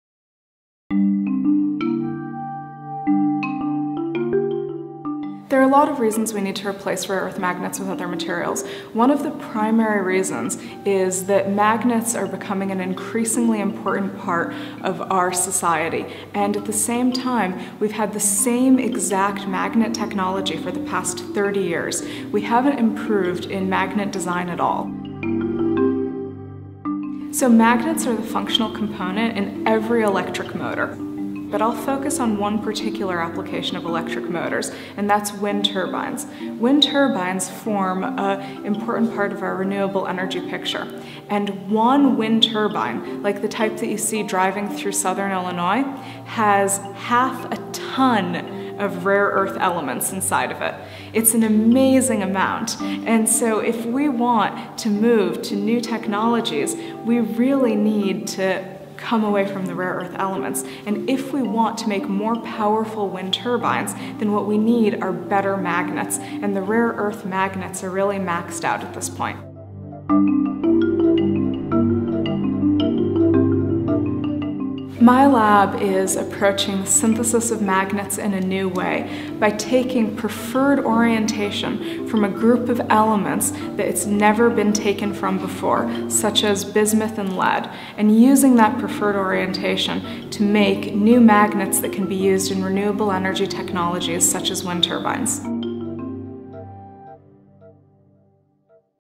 Danna Freedman, Northwestern U.: Fundamental Chemical Discovery for Magnetic Discovery (2018)
Fundamental chemical discovery can enable transformative leaps within functional material applications. Here, we are studying magnetism with the goal of understanding and creating a new class of permanent magnets. Towards that end we are separating a magnetic moment into its two constituent components, spin and orbital angular momentum, housing each in a separate atom and reuniting them to create a powerful magnetic moment. Within this project here is potential for insight into fundamental questions in magnetism. Our primary discovery thus far is the first iron bismuth compound, FeBi2, featuring the first direct Fe-Bi contact in a solid-state material. Dr. Freedman is a 2018 Camille Dreyfus Teacher-Scholar.